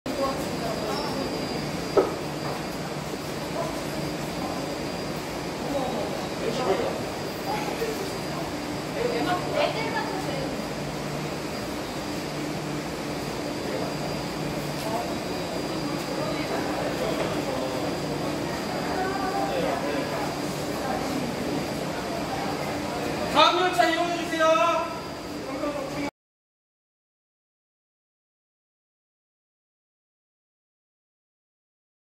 미니 경전철 / 신림선에서 벌어진일 Jalur kereta api ringan Sillim
첨승 직원의 빠른 대처로 이산가족이 발생하지 않았습니다..